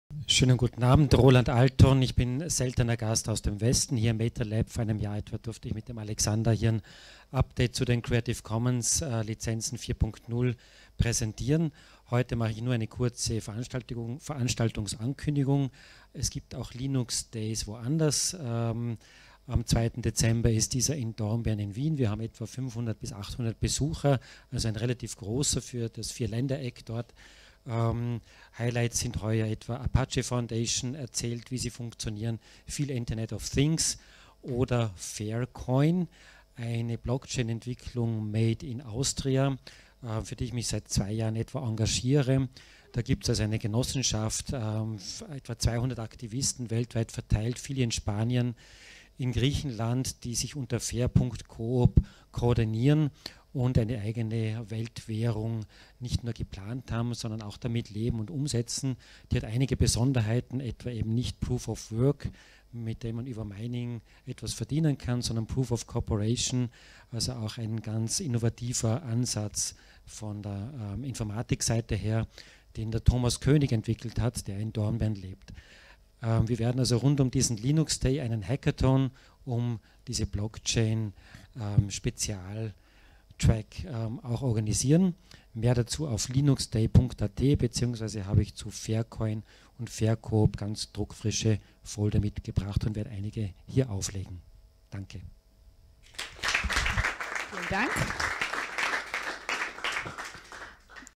20. Netzpolitischer Abend AT 08 Lightning Talk 3 Linux Days 2 Dezember Dornbirn
Lightning Talk 3 Linux Days 2.Dezember Dornbirn
LinuxDay in Vorarlberg - am 2. Dez. 2017 in Dornbirn

Der nächste LinuxDay wird am Samstag, 2. Dezember 2017 wie üblich in der HTL-Dornbirn stattfinden.